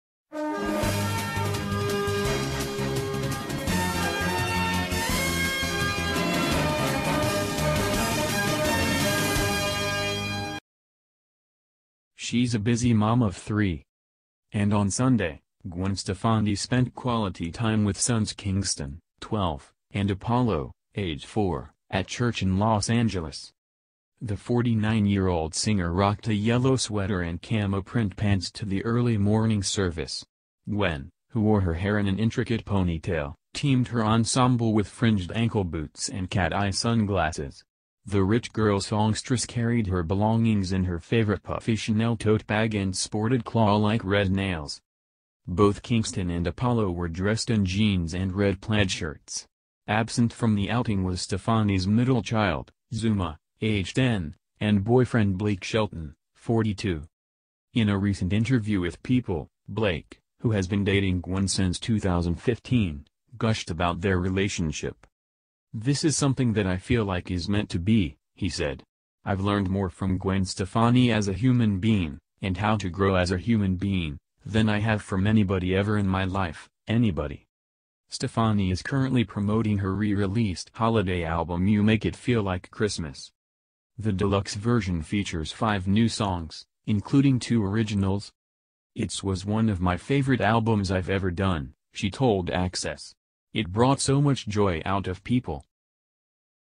Gwen Stefani looks tired as she spends her time with sons Kingston & Apollo at church in Los Angeles
Gwen Stefani stands out in a yellow sweater and camo-print slacks as she takes sons Apollo and Kingston to church